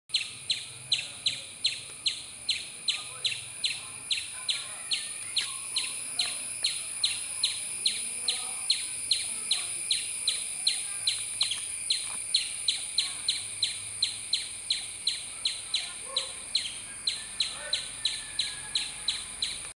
Nikon D 7000 1080p Video + Sigma Macro 105 - Test
Handheld shot of a glassy blue tiger.
Shot with a Nikon D 7000 + Sigma 105mm